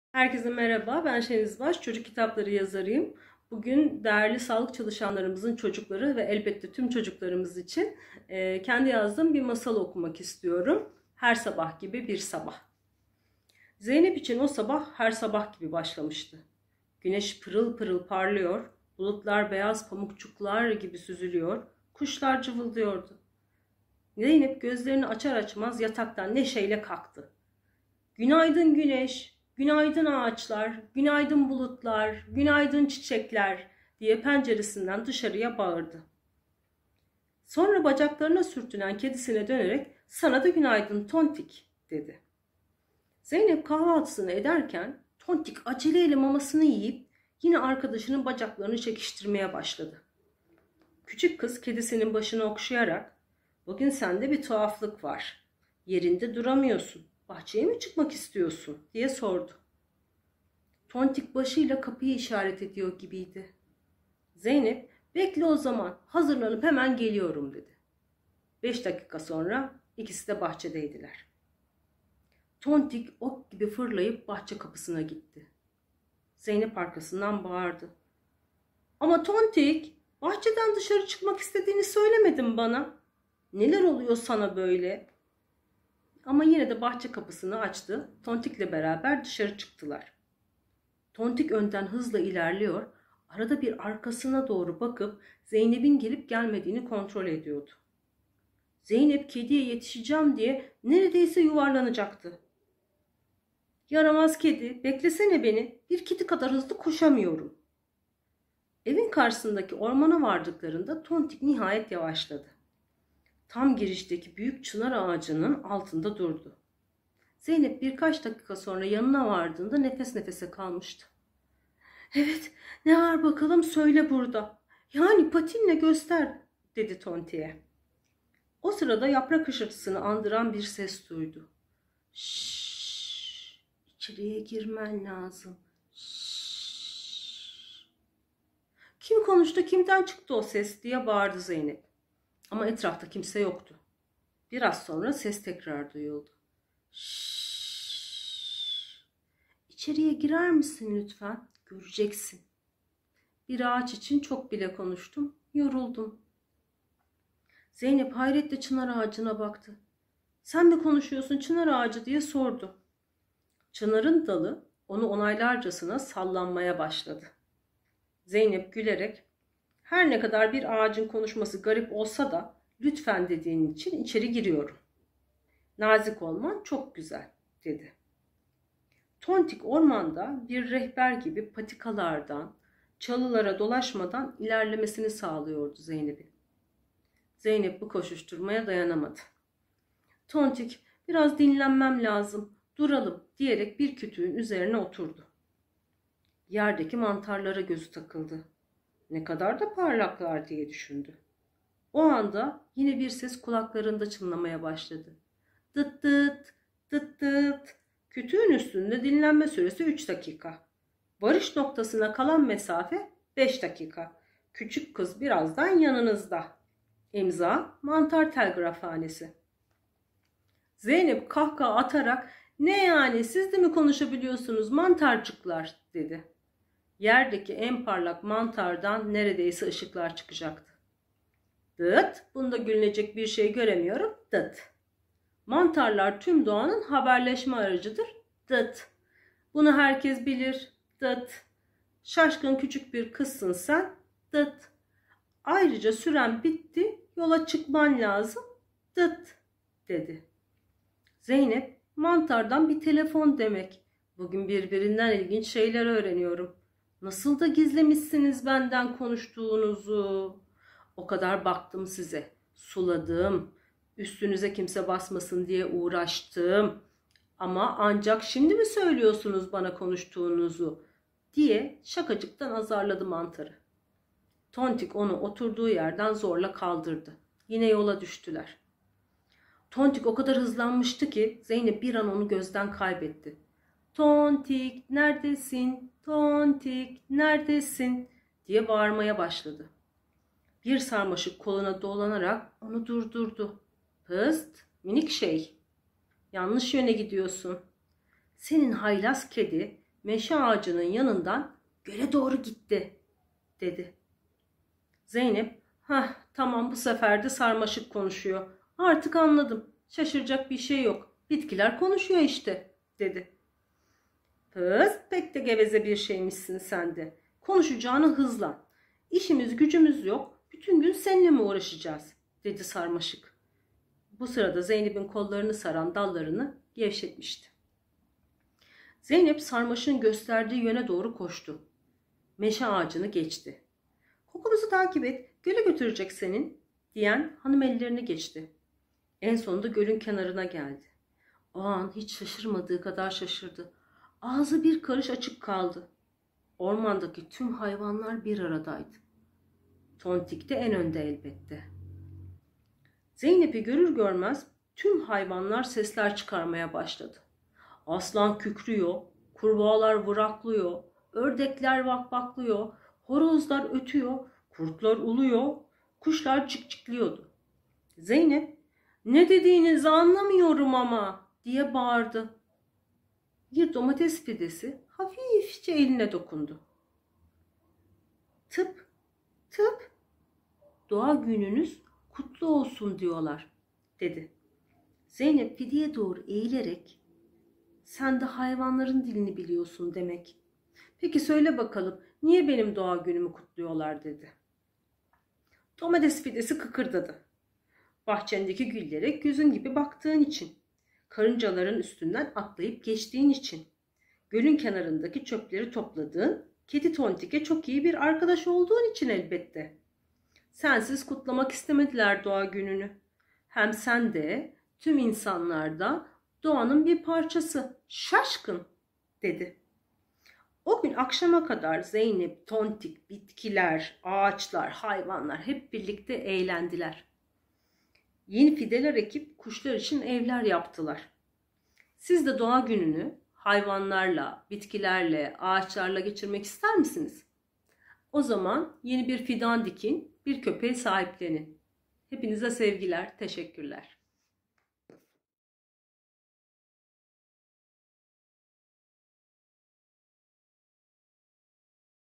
Her Sabah Gibi Bir Sabah (Yazan: Şeniz Baş)
Türkiye Çocuk ve Genç Psikiyatrisi Derneği'nin öncülük ettiği Masal Pandemisi projesinde kitapçı, editör, yayıncı ve çocuk kitapları yazarı Şeniz Baş kendisinin yazdığı,  Her Sabah gibi bir Sabah isimli hikayeyi okudu.